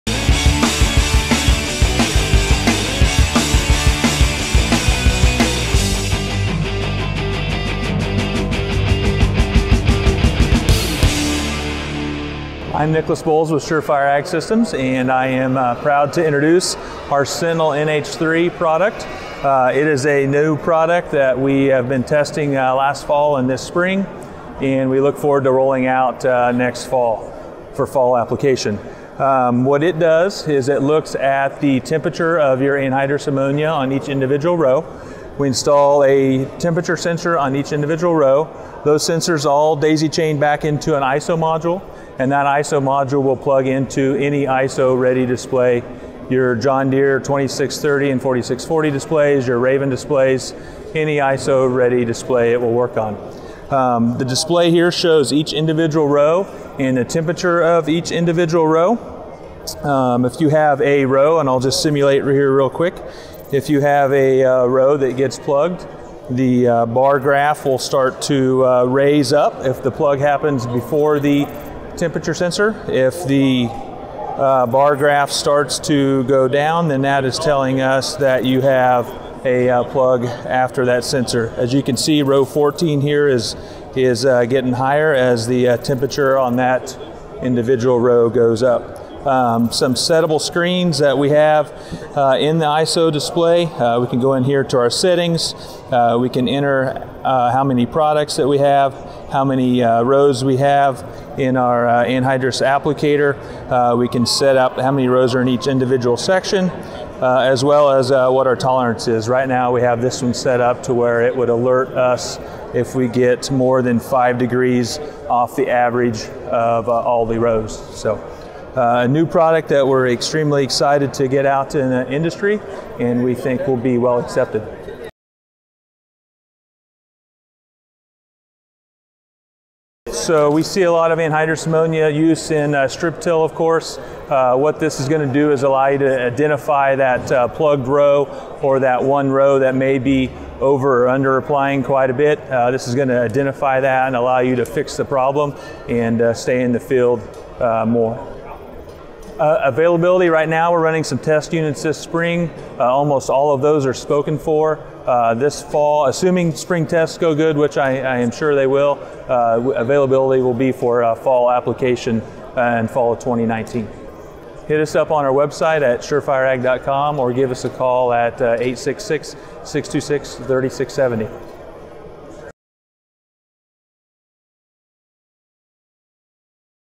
Surefire Ag Launches New Sentinel Anhydrous Row Monitor
Surefire Ag Systems debuts its new Sentinel NH3 row monitor solution, which is ISOBUS compatible and can detect full or partial blockages on each row.